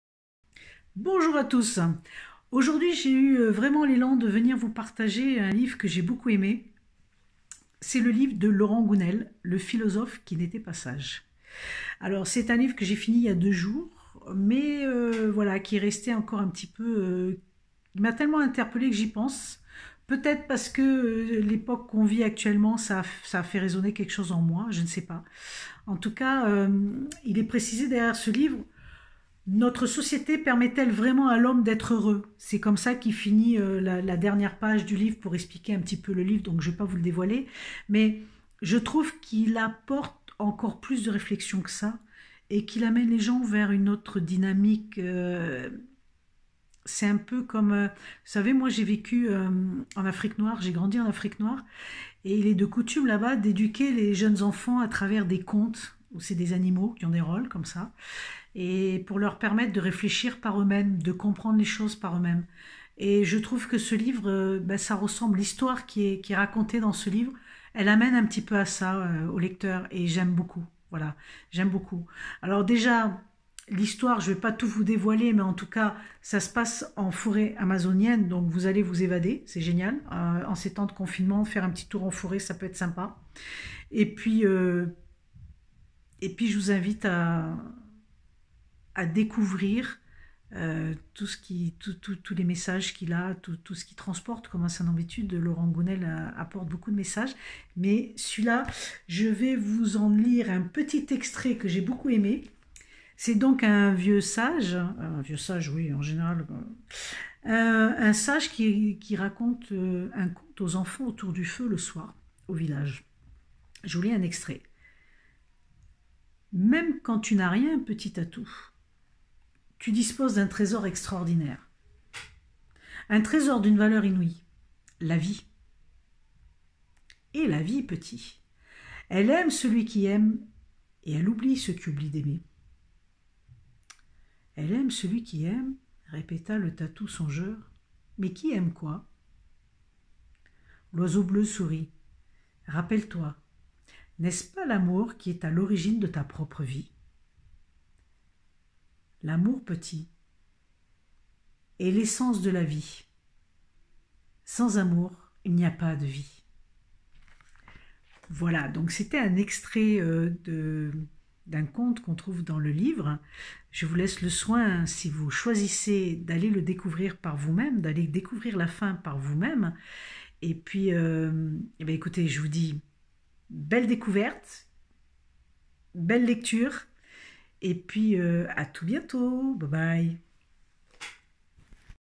LE PHILOSOPHE QUI N'ETAIT PAS SAGE de Laurent Gounelle
la lecture de ce livre vous amené, autre de passer un bon moment, vers une réflexion intéressante sur comment je vous laisse le soin de le découvrir par vous-même.